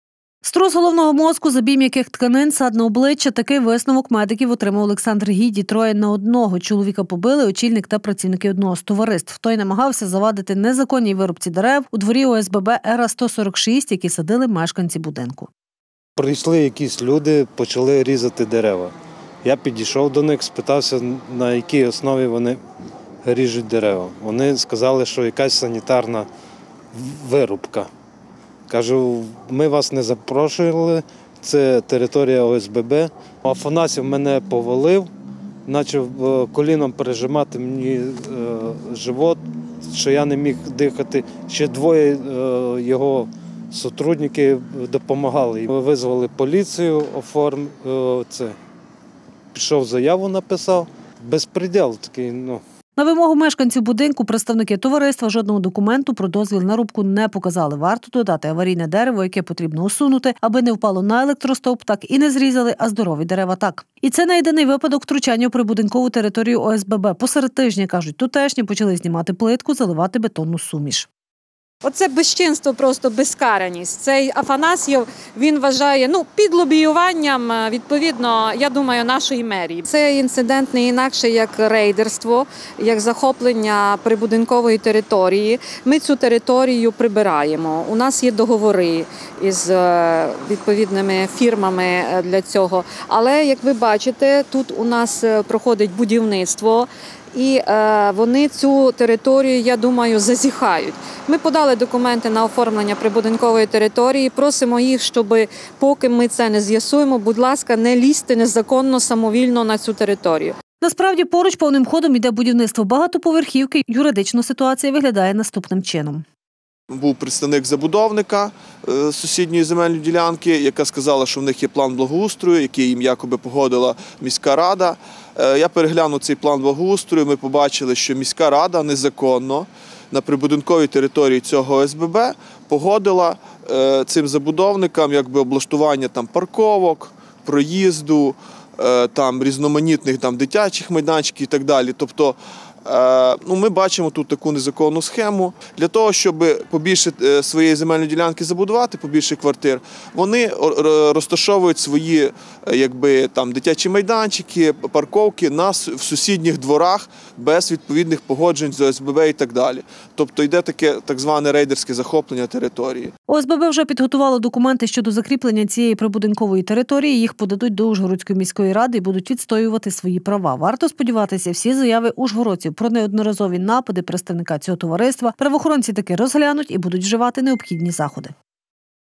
Побиття представників ОСББ. Резонансна справа
Струс головного мозку, забій м’яких тканин, садна обличчя – такий висновок медиків отримав Олександр Гіді. Троє на одного - чоловіка побили очільник та працівники ТОВ "Наш-Добробут". Той намагався завадити незаконній вирубці дерев у дворі ОСББ «Ера,146», які садили мешканці будинку. На вимогу мешканців будинку представники ТОВ «Наш добробут» жодного документу про дозвіл на вирубку тут дерев не показали! Варто додати, аварійне дерево, яке потрібно усунути, аби не впало на електротовп – так і не зрізали, а здорові дерева позрізали! І це не єдиний випадок втручання у прибудинкову територію ОСББ – серед тижня, кажуть тутешні, почали знімати плитку, заливати бетонну суміш.